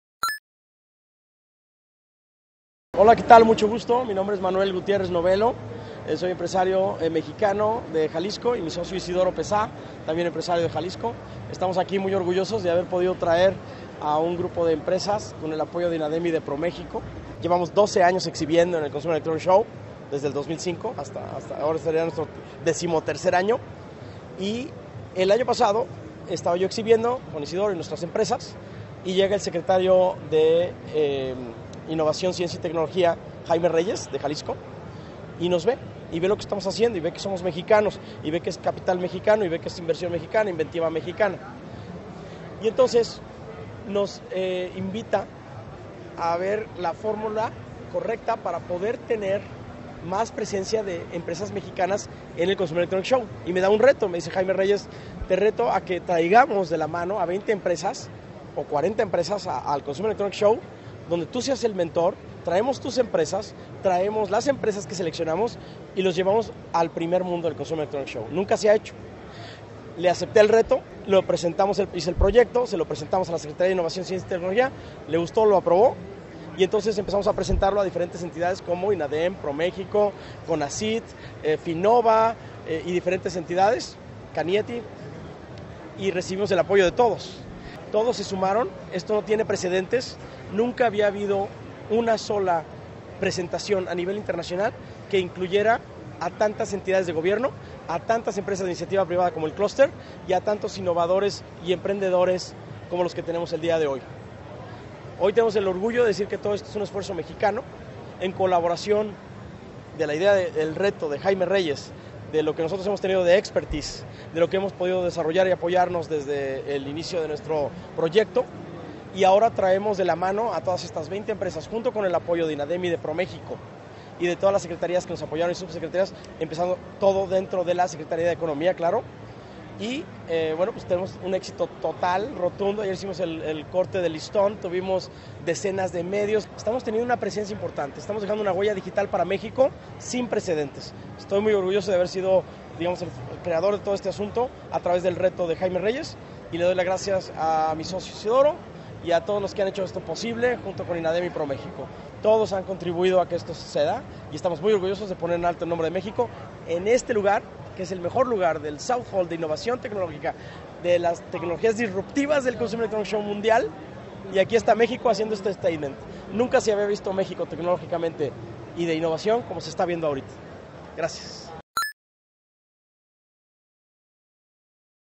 Manuel Gutiérrez Novelo en CES 2018
Manuel Gutiérrez habla de cómo cobró forma el proyecto del pabellón de México en el CES 2018